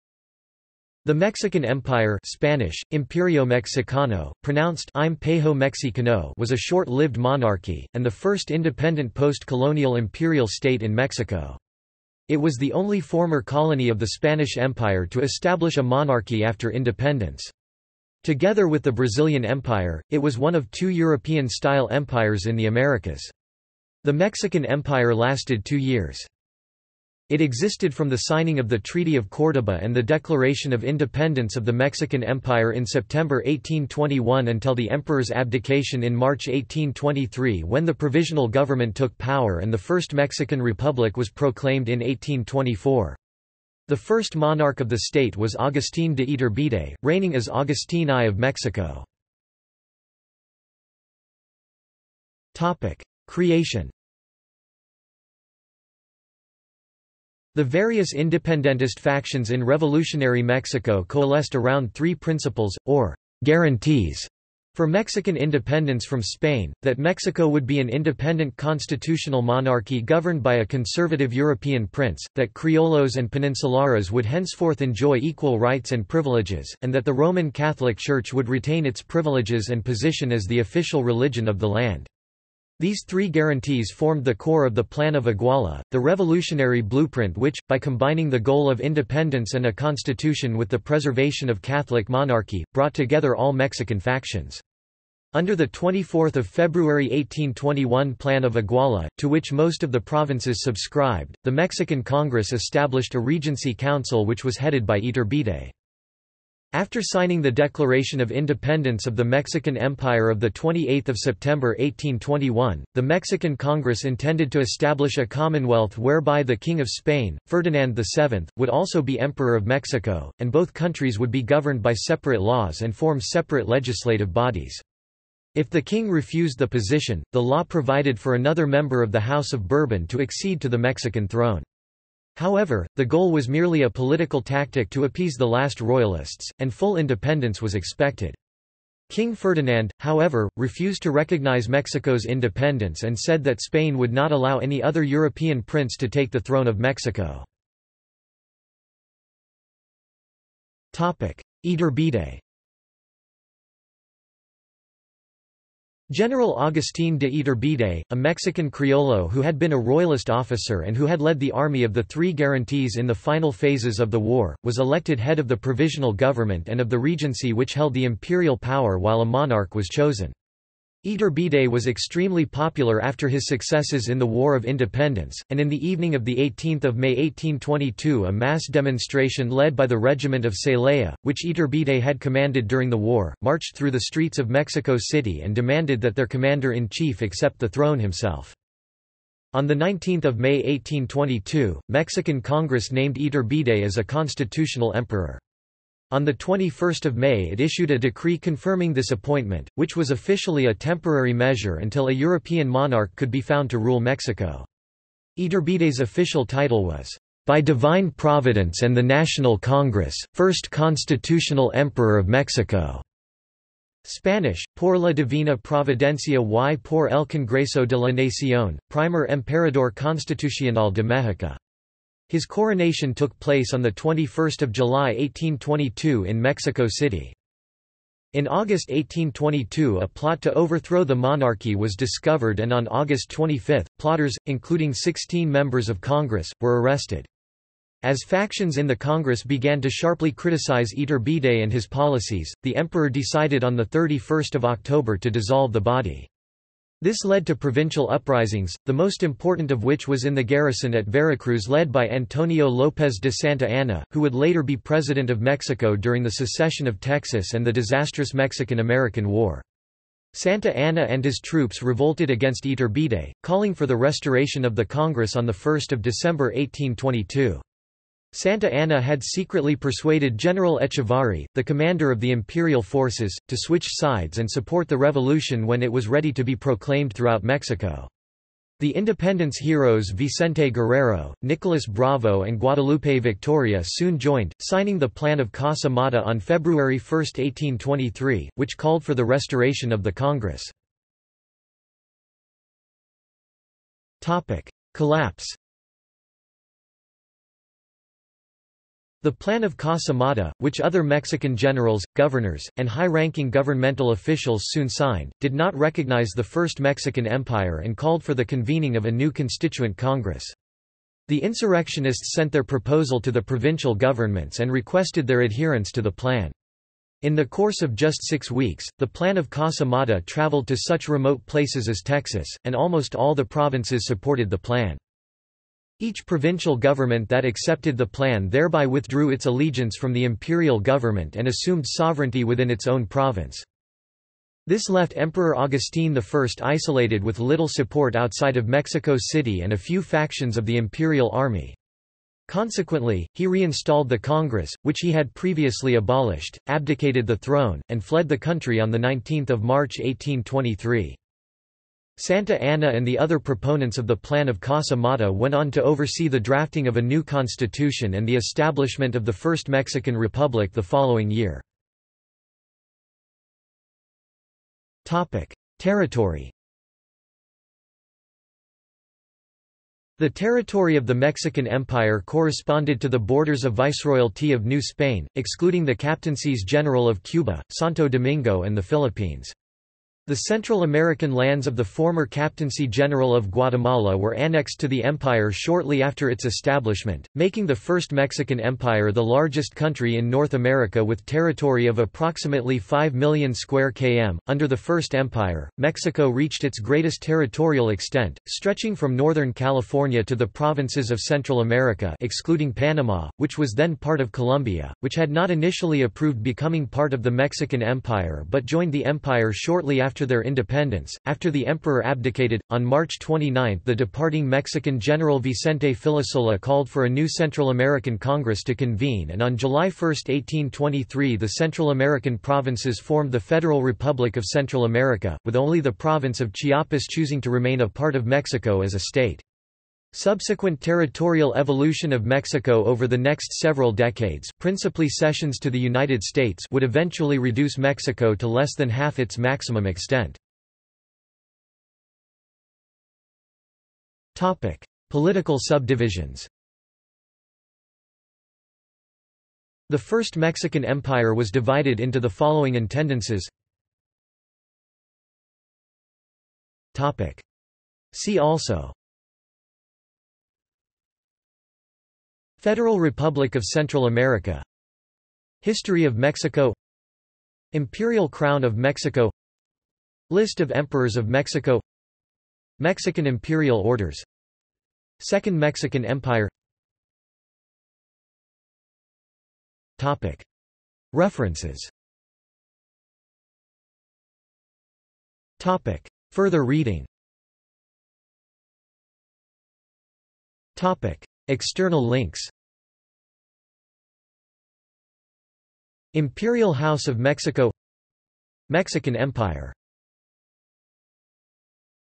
First Mexican Empire | Wikipedia audio article
This is an audio version of the Wikipedia Article:
First Mexican Empire

- Socrates

SUMMARY
The Mexican Empire (Spanish: Imperio Mexicano, pronounced [ĩmˈpeɾjo mexiˈcano]) was a short lived monarchy, and the first independent post-colonial imperial state in Mexico. It was the only former colony of the Spanish Empire to establish a monarchy after independence. Together with the Brazilian Empire, it was one of two European-style empires in the Americas. The Mexican Empire lasted two years.
It existed from the signing of the Treaty of Córdoba and the declaration of Independence of the Mexican Empire in September 1821 until the emperor's abdication in March 1823 when the Provisional Government took power and the First Mexican Republic was proclaimed in 1824. The first monarch of the state was Agustín de Iturbide, reigning as Agustín I of Mexico.